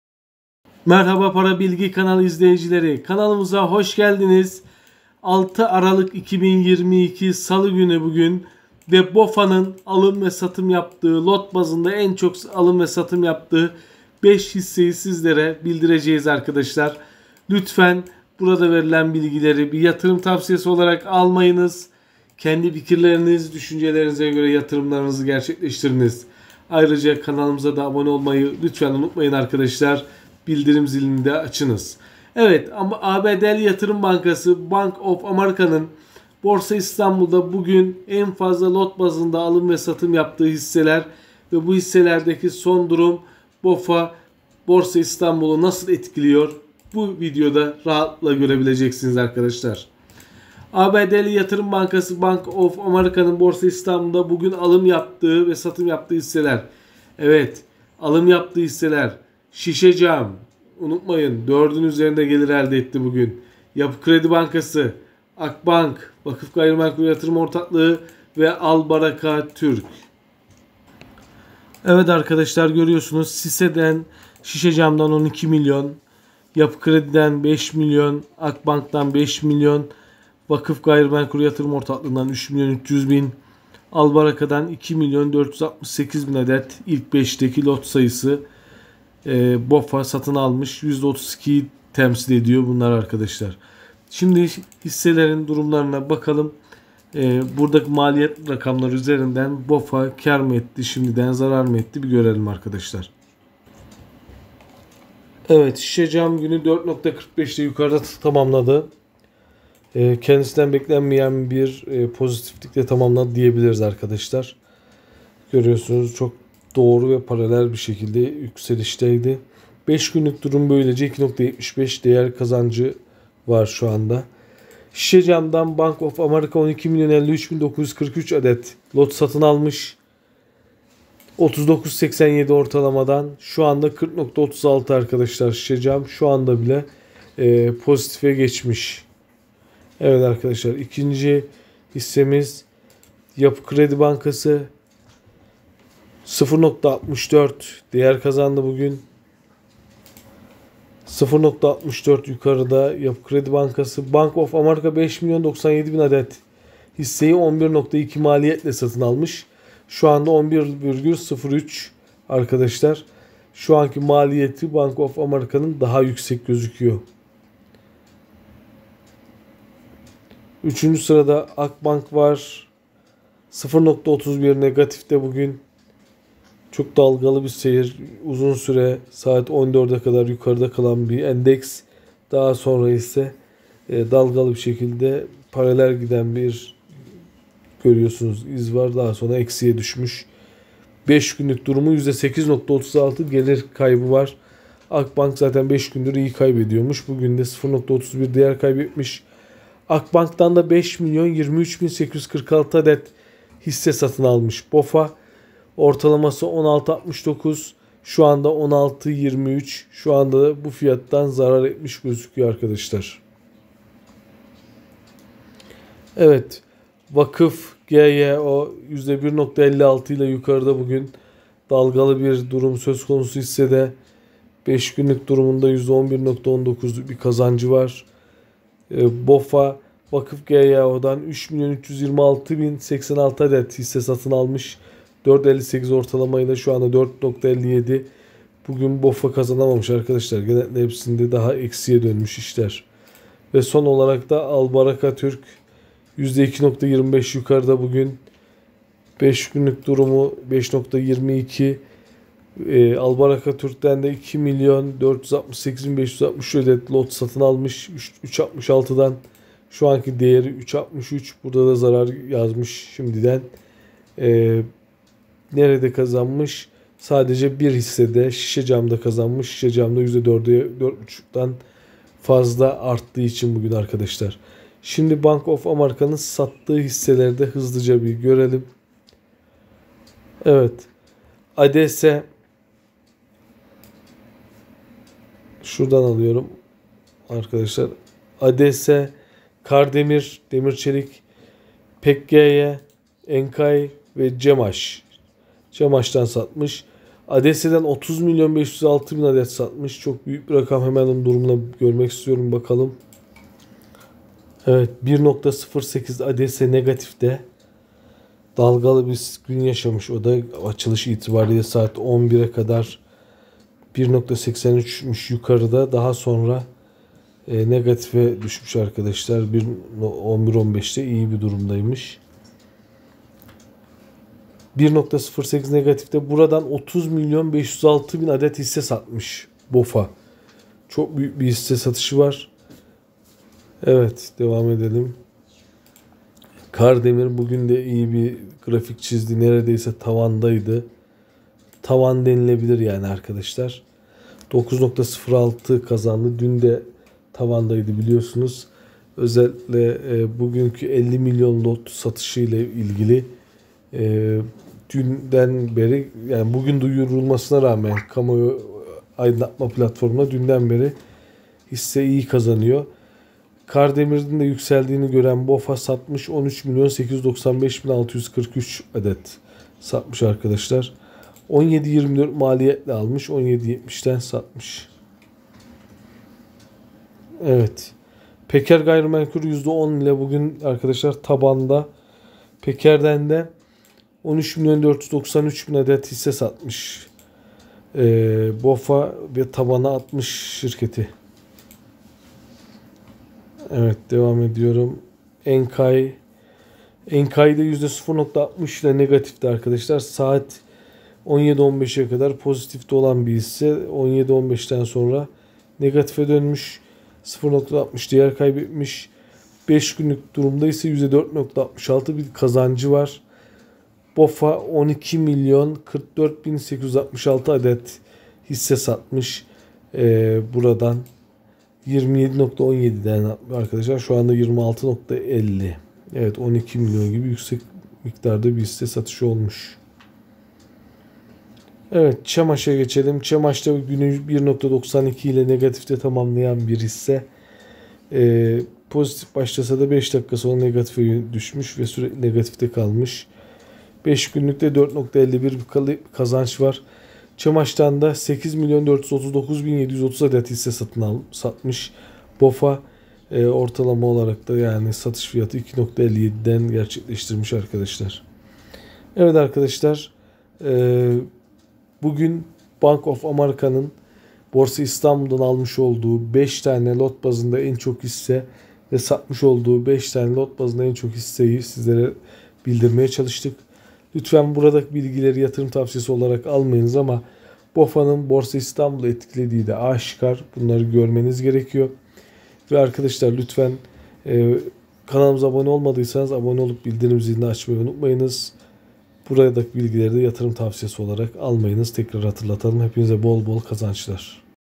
Bank Of Amerika'nın Bugün En Fazla Satın Aldığı ve Sattığı Hisseler!
Bank Of Amerika'nın 6 Kasım 2022 tarihinde en fazla satın aldığı 5 hisse ve sattığı 5 hisse. İşte Detaylar...